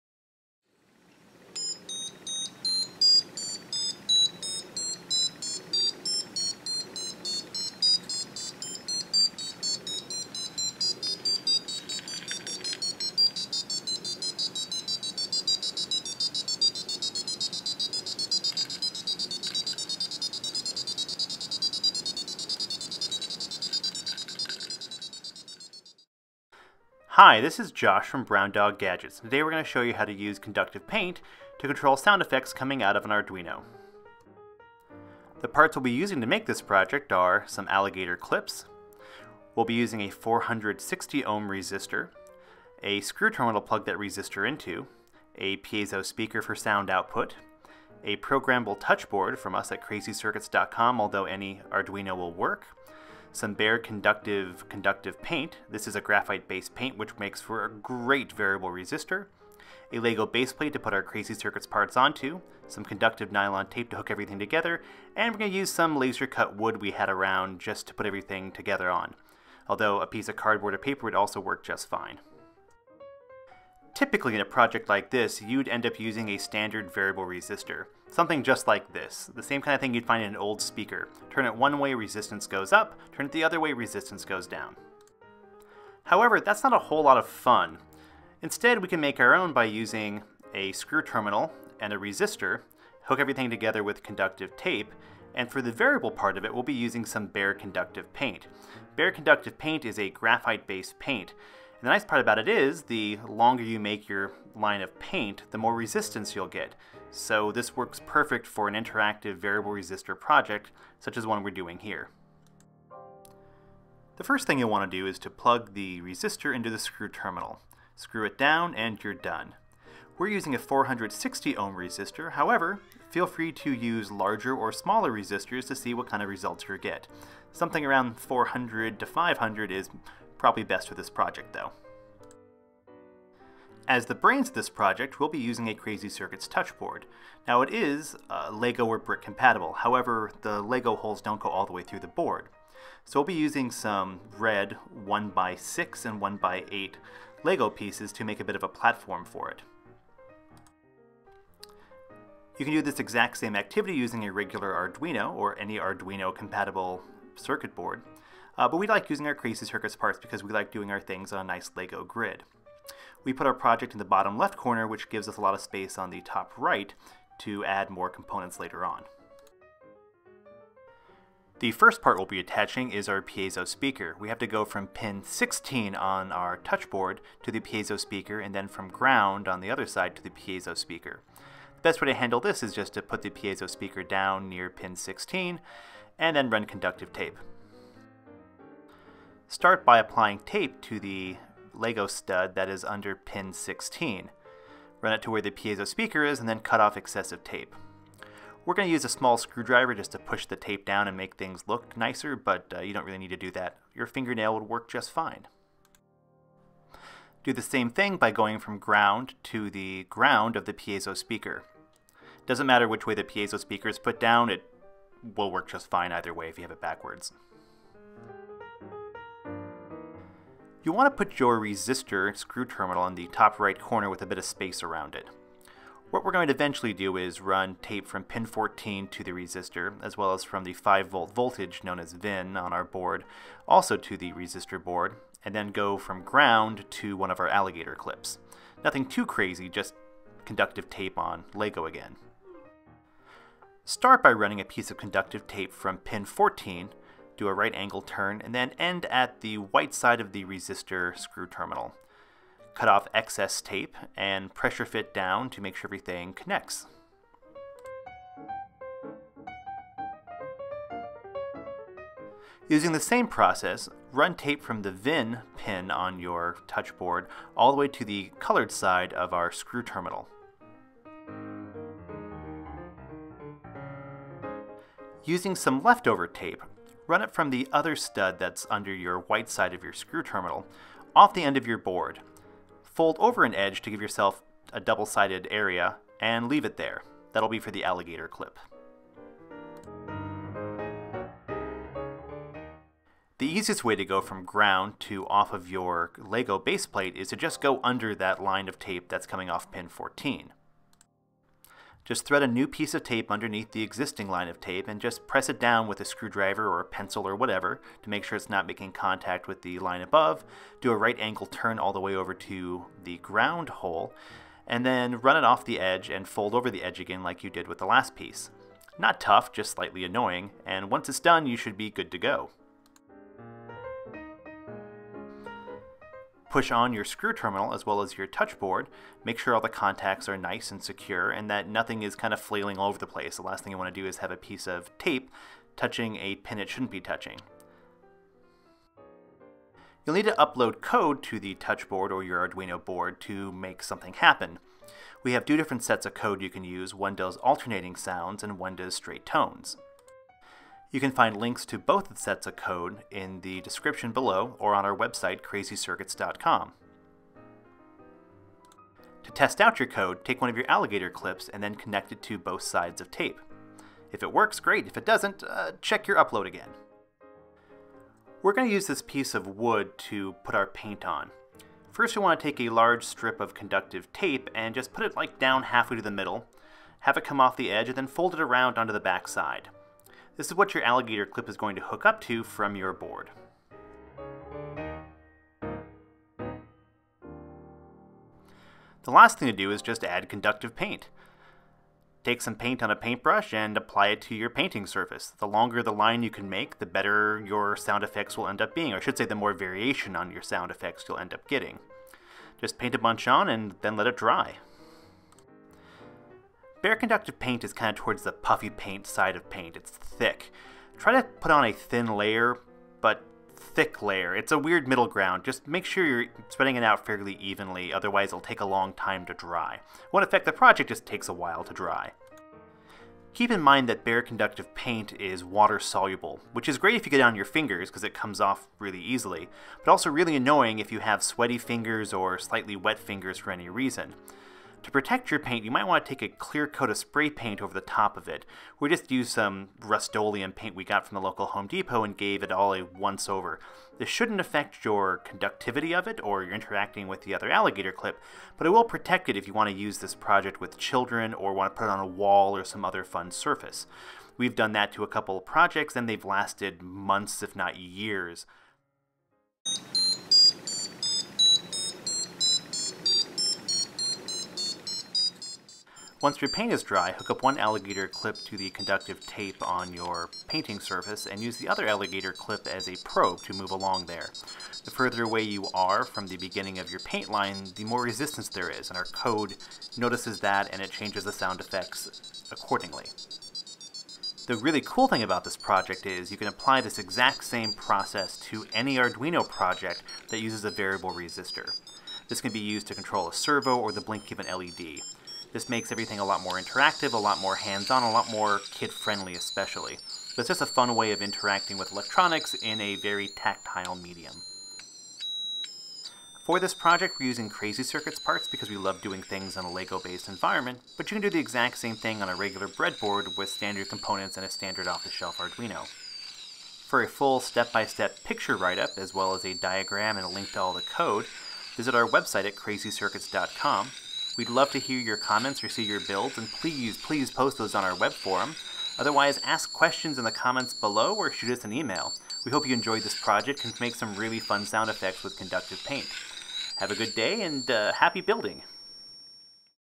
Conductive Paint and Arduino Sound Effects!
Use conductive paint to create fun sound effects coming off an Arduino.